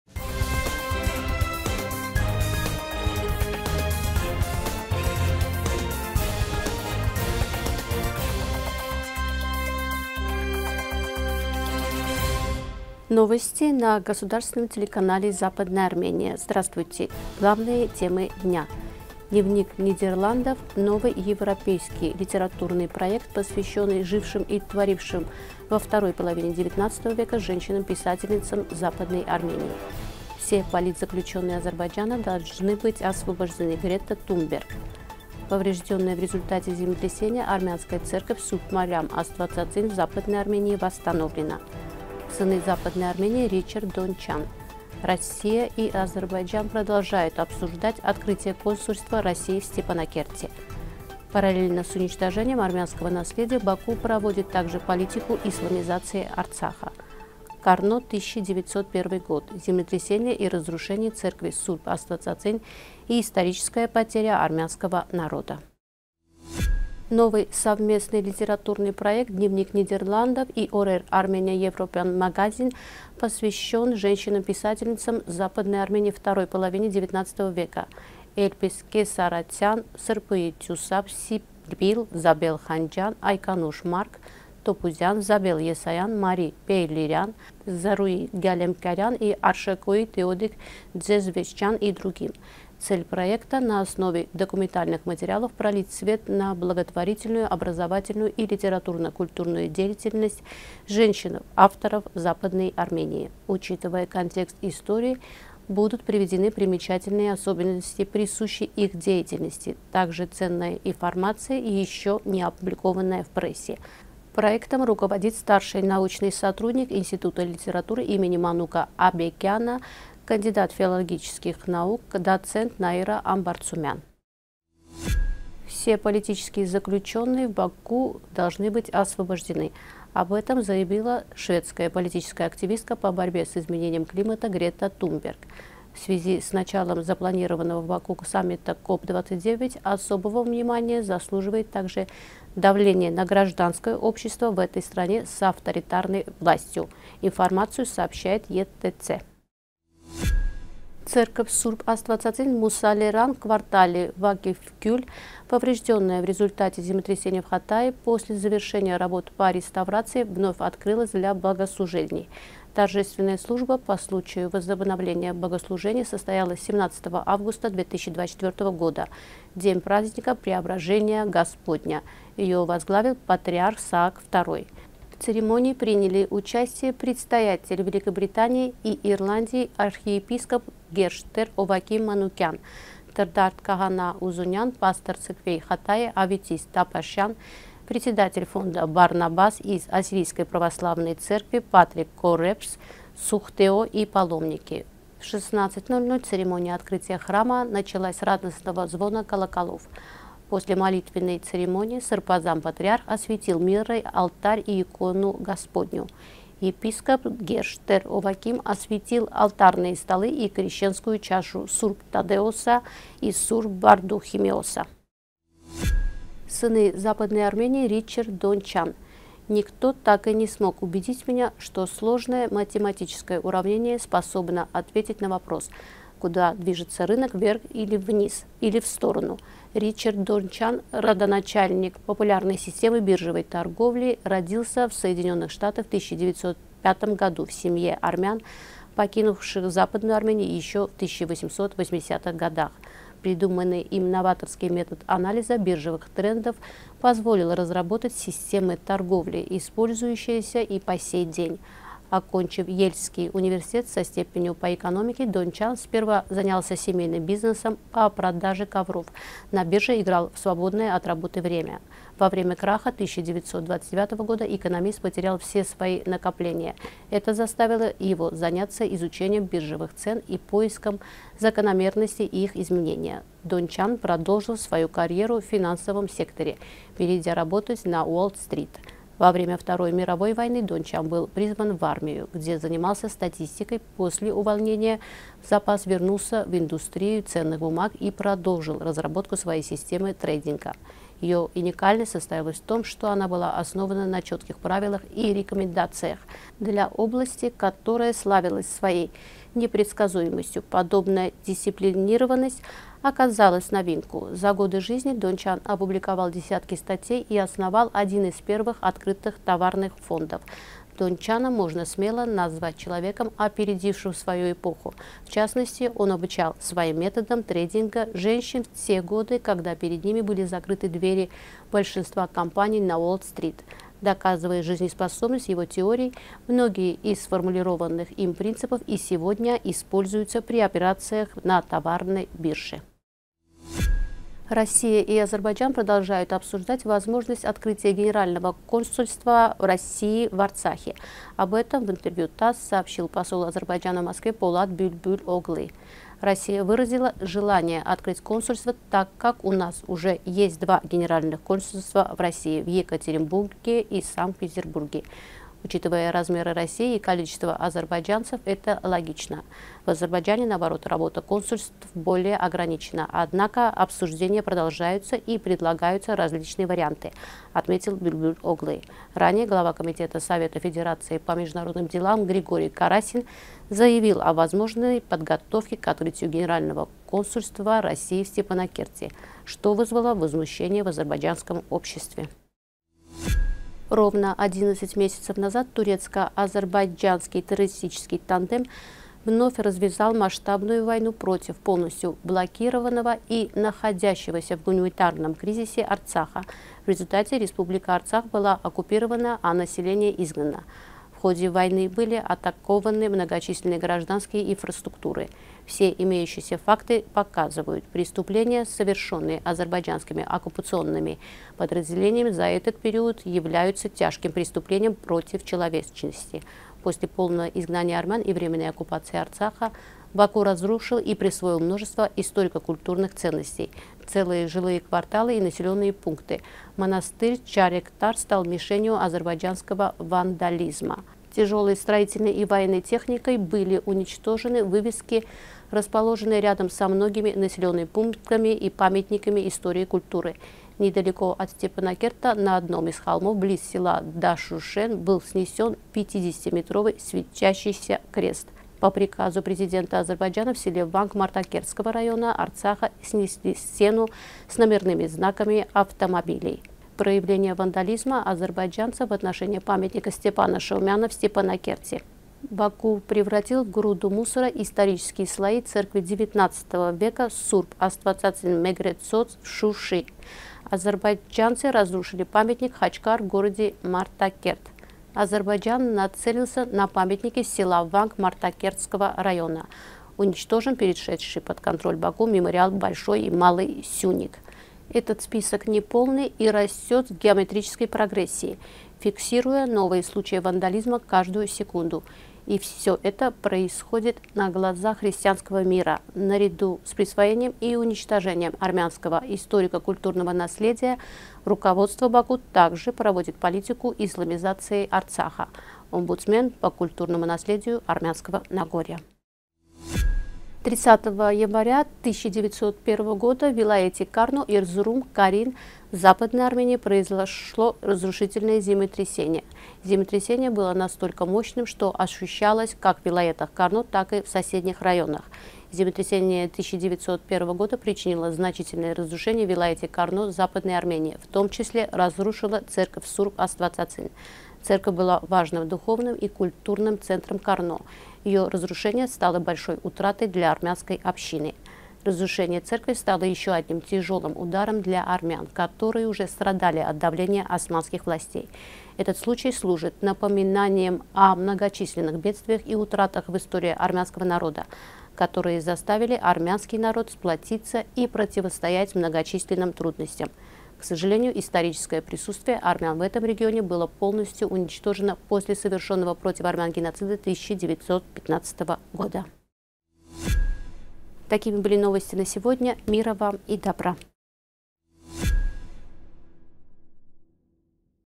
Параллельно с уничтожением армянского наследия Баку проводит также политику исламизации Арцаха
1. Дневник Нидерландов/ новый европейский литературный проект, посвященный жившим и творившим во второй половине 19-го века женщинам писательницам Западной Армении

2. Все политзаключенные Азербайджана должны быть освобождены:Грета Тунберг

3. Поврежденная в результате землетрясения армянская церковь  Сурб Мариам Аствацацин в Западной Армении восстановлена

4. Сыны Западной Армении:Ричард Дончян

5. Россия и Азербайджан продолжают обсуждать открытие консульства России в Степанакерте

6. Параллельно с уничтожением армянского наследия Баку проводит также политику исламизации Арцаха

7. "Карно 1901 год:землетрясение и разрушение церкви Сурб Аствацацин и историческая потеря армянского народа”